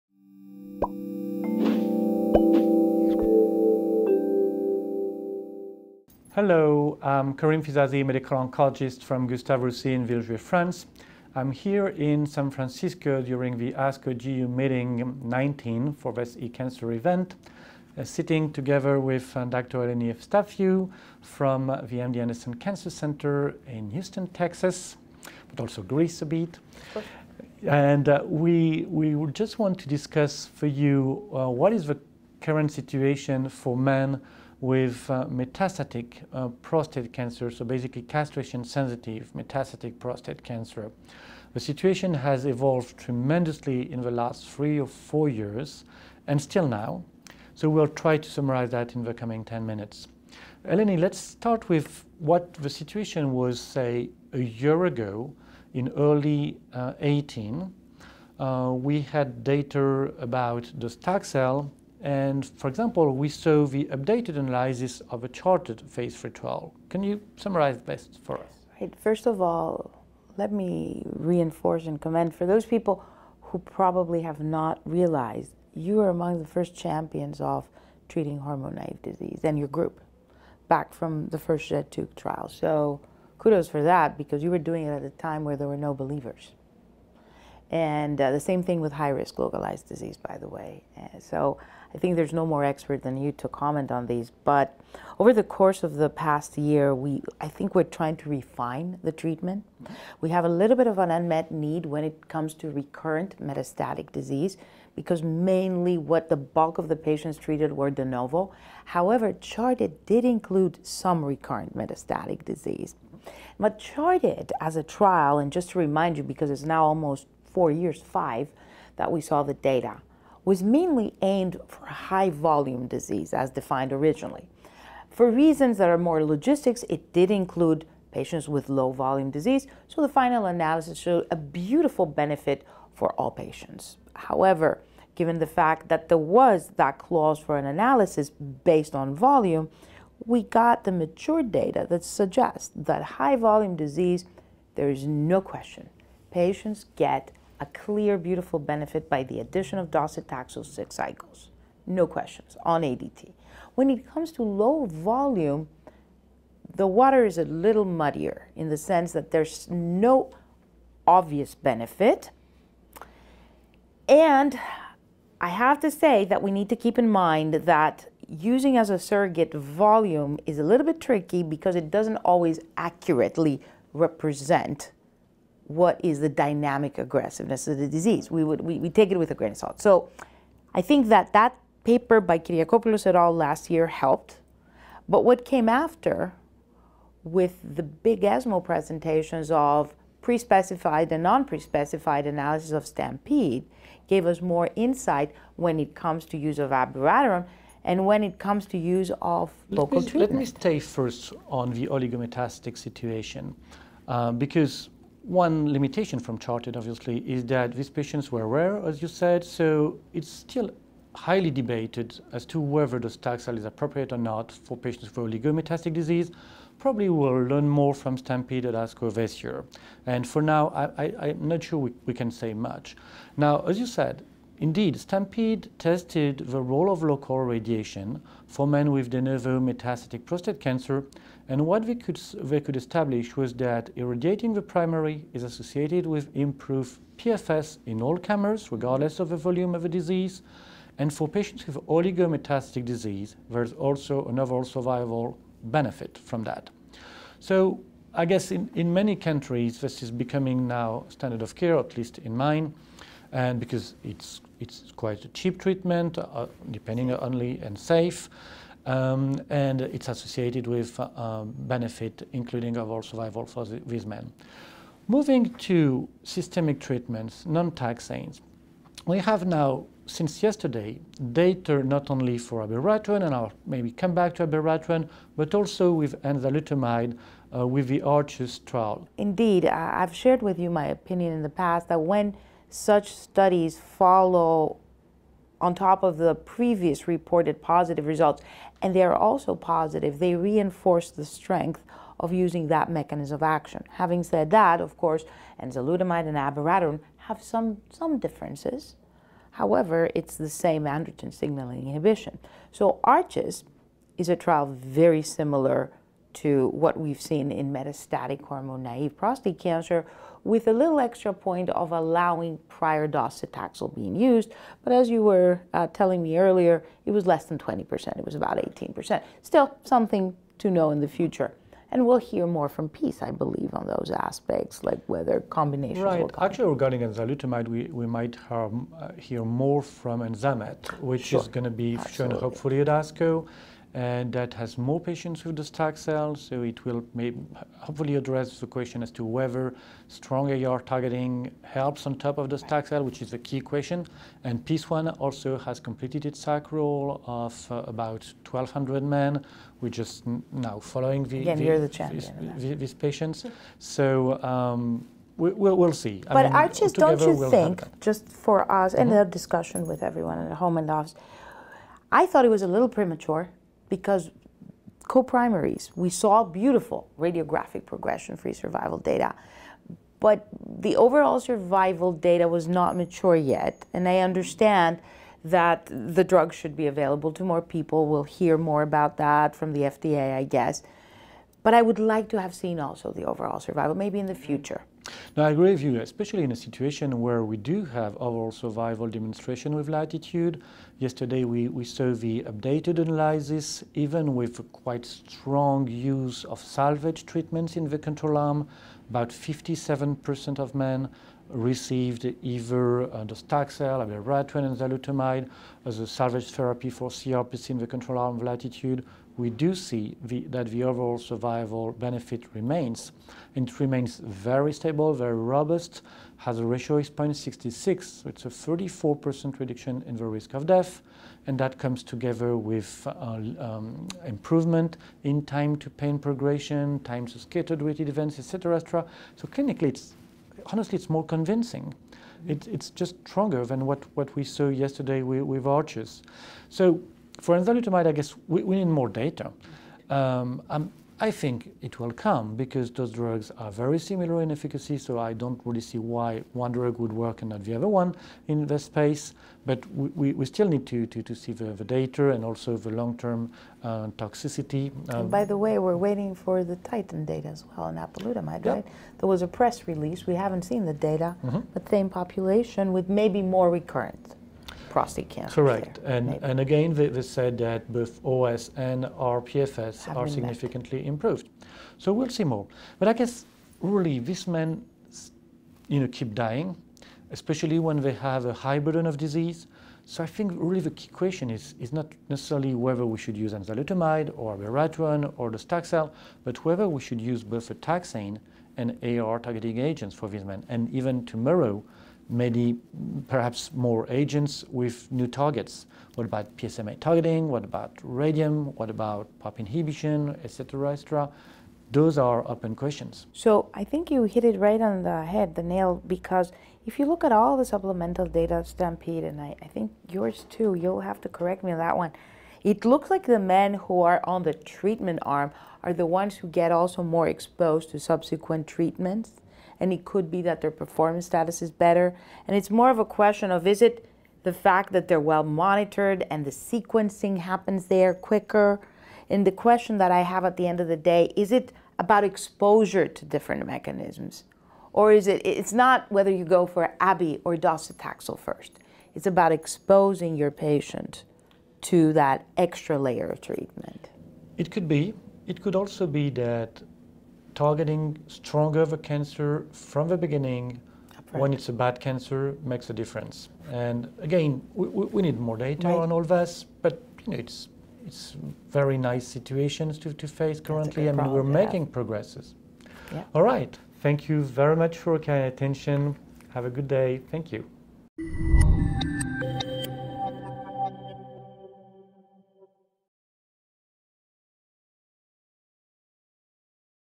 ASCO GU 2019: treatment and management of hormone-sensitive metastatic prostate cancer
Professor Karim Fizazi and Professor Eleni Efstathiou set the scene in current care and discuss the treatment options available for hormone-sensitive metastatic prostate cancer, highlighting the unmet need in recurrent metastatic disease.

New updates to the outcomes of the STAMPEDE, ARCHES, and LATITUDE studies presented at ASCO GU 2019 are also reviewed.

Finally, Professors Fizazi and Efstathiou consider the need for longer term efficacy and safety data for enzalutamide.

This programme has been supported by an unrestricted educational grant from Janssen Pharmaceutica (A Johnson & Johnson Company).